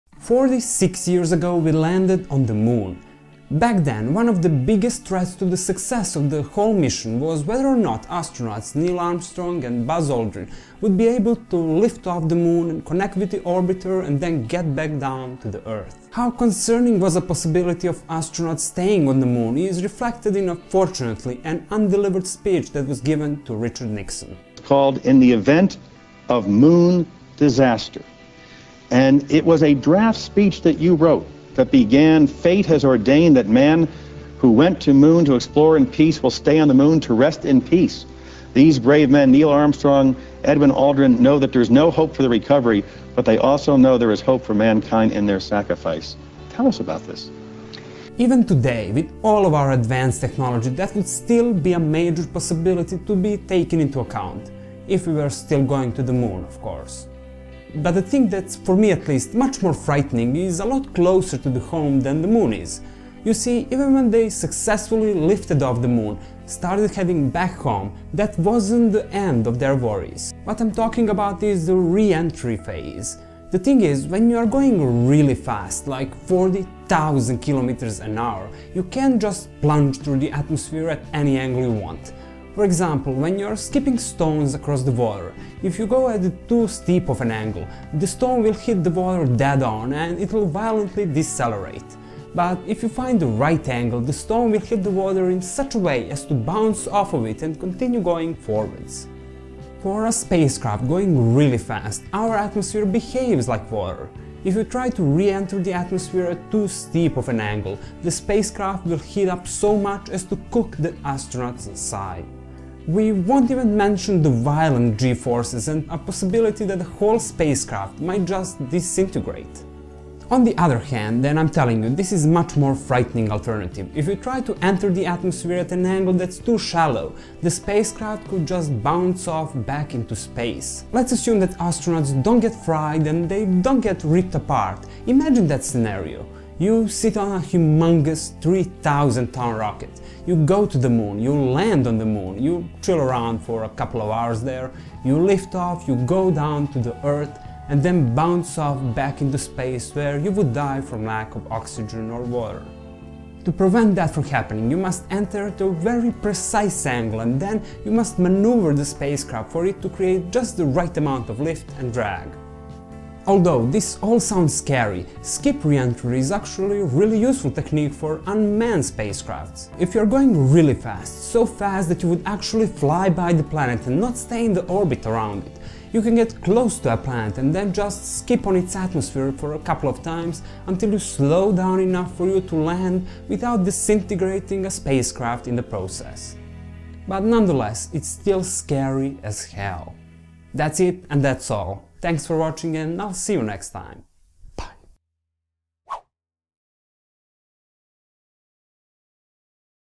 Astronauts bouncing off Earth!
46 years ago humans have first set foot on another world. Among all of the things that could go wrong, this one ain't well known today but it's probably the most frightening one.

Sources:

Coming tomorrow.